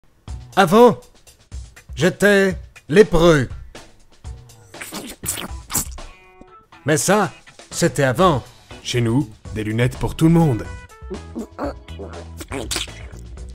Lepreux
Spot vidéo réalisé pour le spectacle "Carte Blanche : Les Betamax sont des garçons hilarants". Un spectacle one-shot pour fêter les 5 ans du trio Betamaxxx.